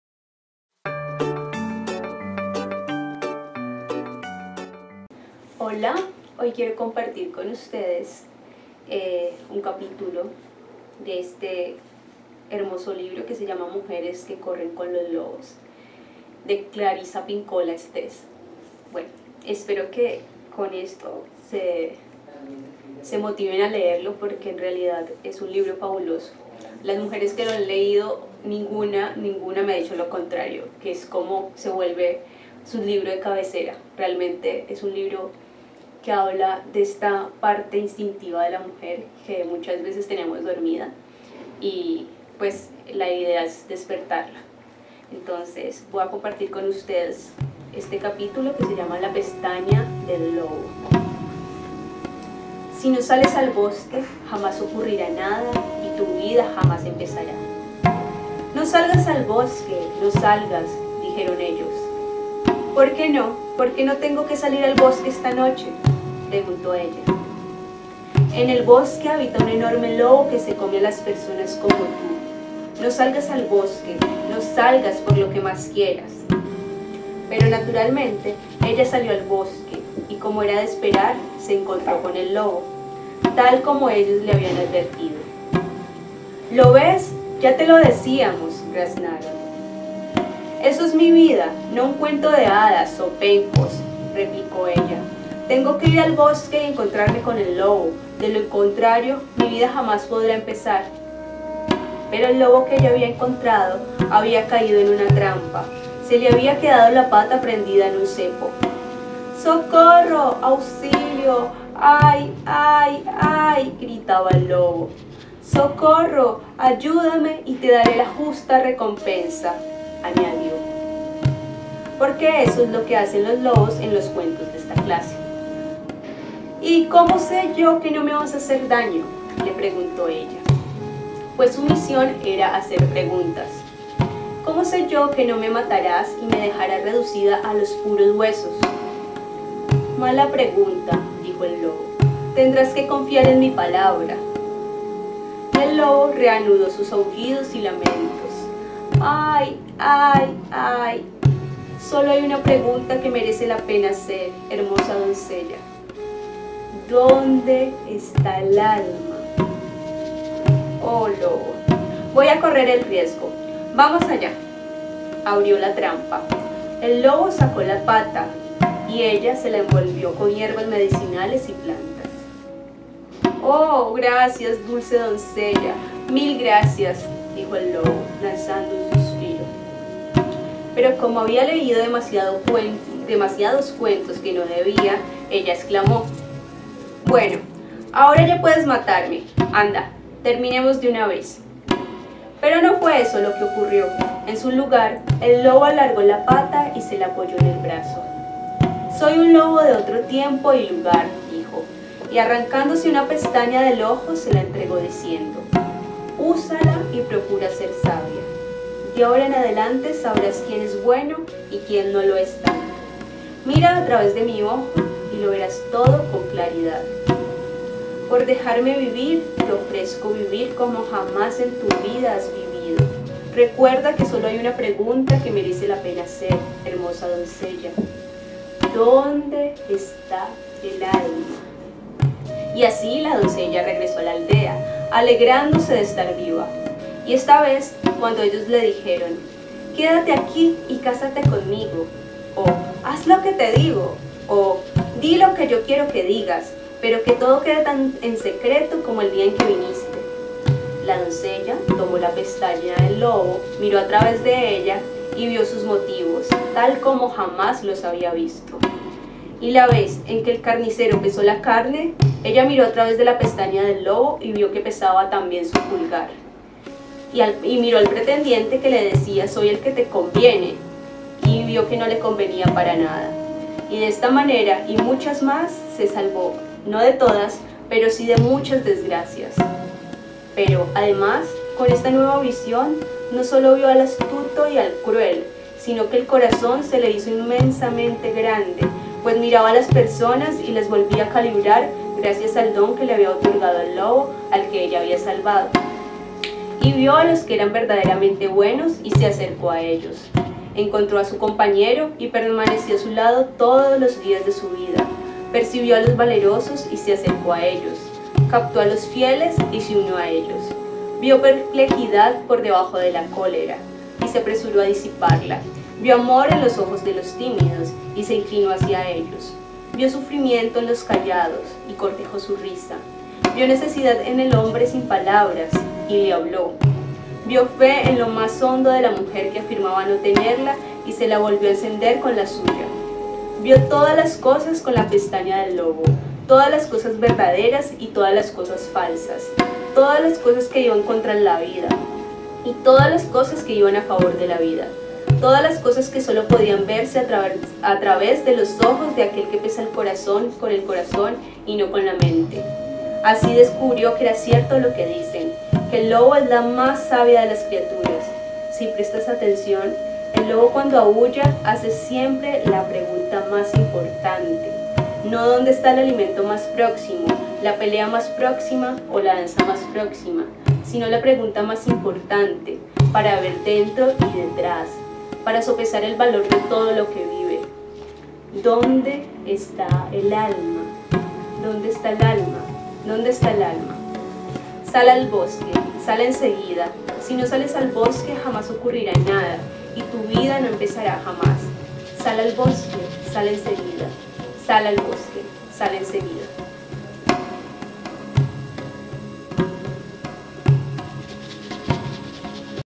Mujeres que corren con los lobos- La pestaña del lobo
Todos sentimos el anhelo de lo salvaje. Y este anhelo tiene muy pocos antídotos culturalmente aceptados. Nos han enseñado a avergorzarnos de este deseo. Nos hemos dejado el cabello largo y con él ocultamos nuestros sentimientos. Pero la sombra de la mujer salvaje acecha todavía a nuestra espalda de día y de noche. Dondequiera que estemos, la sombra que trota detrás de nosotros tiene sin duda cuatro patas.

Clarissa Pinkola Estés